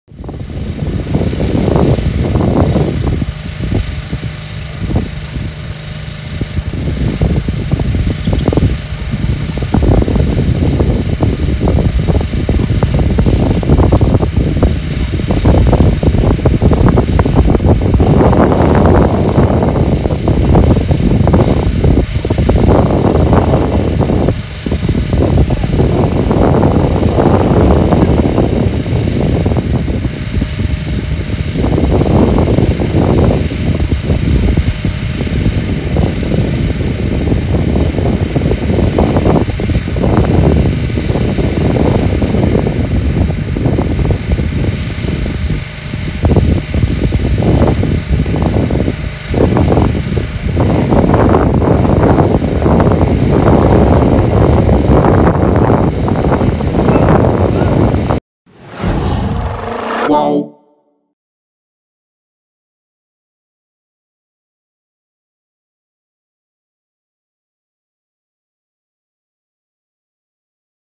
Trégua entre Hamas e Israel: Grupo libera 13 reféns israelenses à Cruz Vermelha no 2º dia
O grupo Hamas entregou a equipes da Cruz Vermelha mais 13 reféns israelenses em troca de 39 palestinos mantidos em Israel. Também foram liberados estrangeiros, mas há divergência sobre a quantidade. Este é o segundo dia de trégua na guerra Israel-Hamas